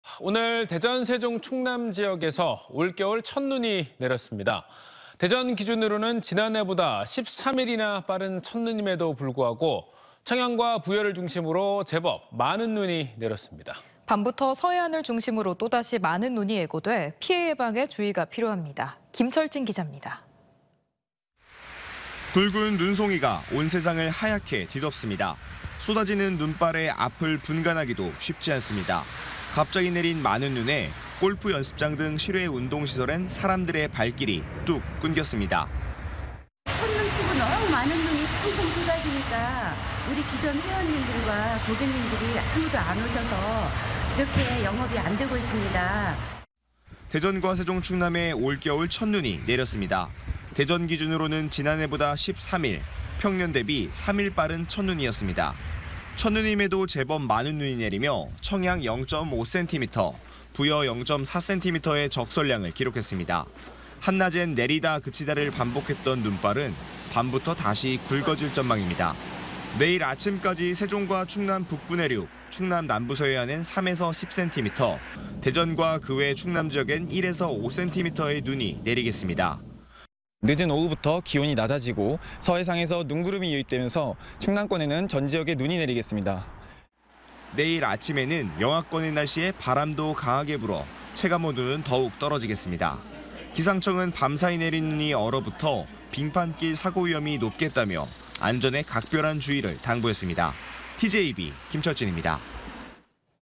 일찍 찾아온 올 겨울 '첫 눈'.. 내일까지 계속｜ TJB 대전·세종·충남뉴스
【 앵커멘트 】

오늘 대전,세종,충남 지역에서
올 겨울 첫눈이 내렸습니다.

대전 기준으로는 지난해보다
13일이나 빠른 첫 눈임에도
청양과 부여를 중심으로
제법 많은 눈이 내렸습니다.

밤부터 서해안을 중심으로
또 다시 많은 눈이 예고돼
피해 예방에 주의가 필요합니다.

김철진 기자입니다.